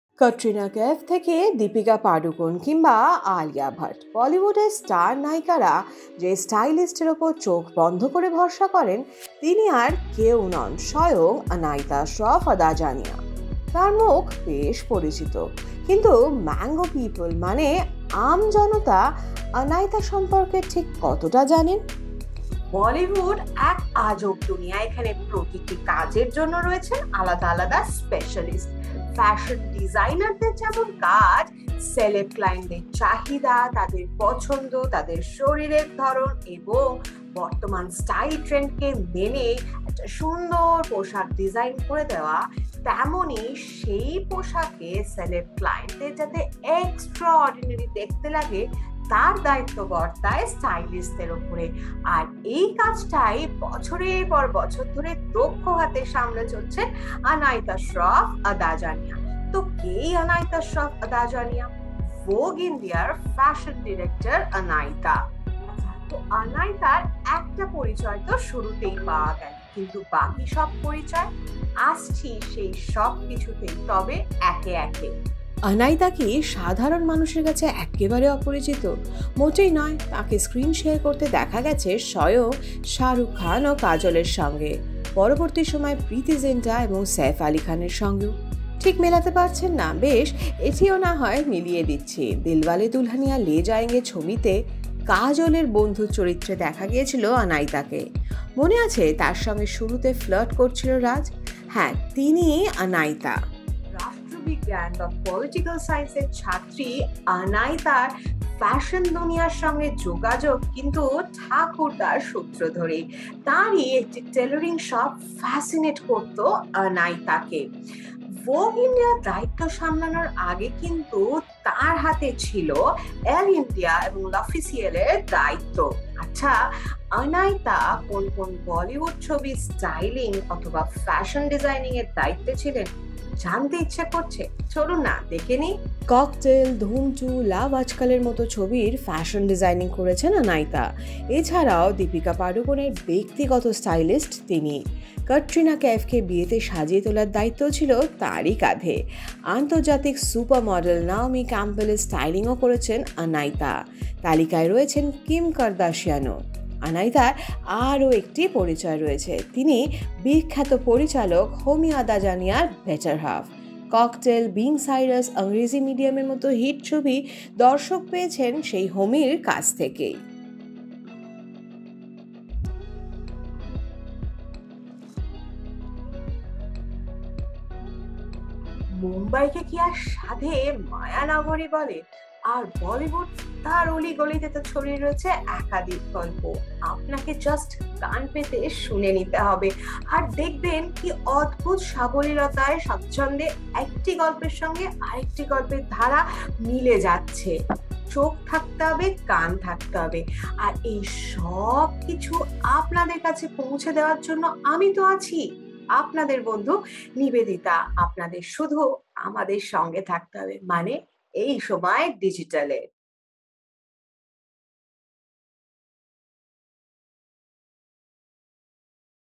বলি স্টারদের প্রিয় স্টাইলিস্ট আনাইতাকে চেনেন?| Bollywood Fashion| Anaita Shroff Adajania
আনাইতা কি সাধারণ মানুষের কাছে এক্কেবারে অপরিচিত? মোটেই নয়। তাঁকে স্ক্রিন শেয়ার করতে দেখা গেছে স্বয়ং শাহরুখ খান ও কাজলের সঙ্গে। পরবর্তী সময়ে প্রীতি জিন্টা এবং সইফ আলি খানের সঙ্গেও!  ঠিক মেলাতে পারছেন না? বেশ এটিও না হয় মিলিয়ে দিচ্ছি। দিলওয়ালে দুলহানিয়া লে জায়েঙ্গে ছবিতে কাজলের বন্ধুর চরিত্রে দেখা গিয়েছিল আনাইতাকে। মনে আছে যাঁর সঙ্গে শুরুতে ফ্লার্ট করছিল রাজ? হ্যাঁ, তিনিই আনাইতা।